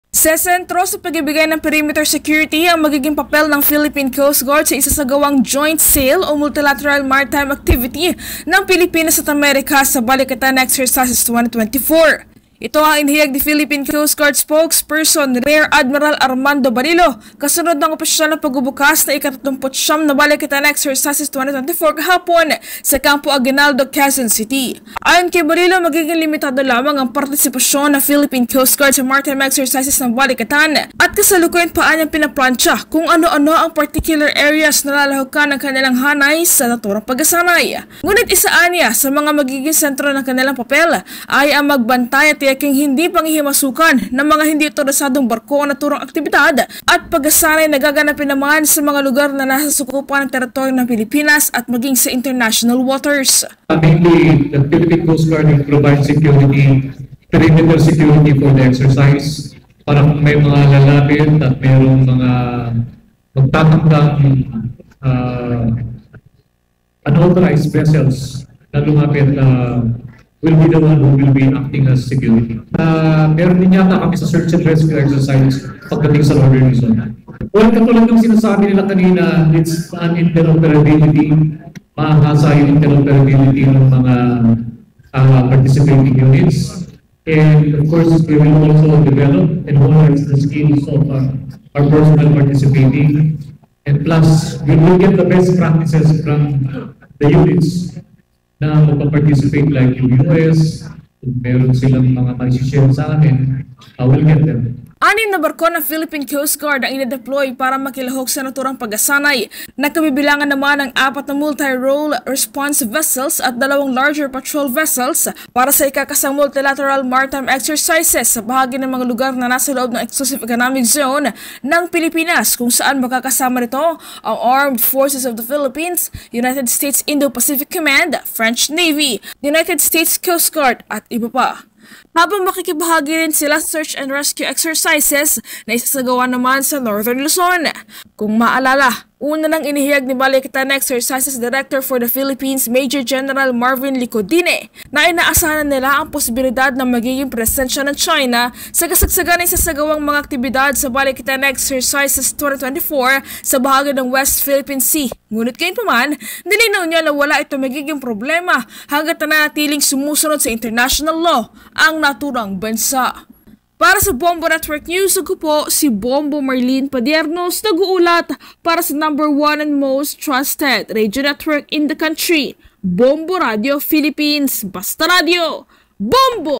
Partisipasyon ng PCG sa Balikatan Exercises 2024, sesentro sa pagbibigay ng 'perimeter security'
Partisipasyon ng PCG sa joint sail na bahagi ng Balikatan Exercises 2024, sesentro sa pagbibigay ng 'perimeter security' laban sa mga presensya ng hindi awtorisadong barko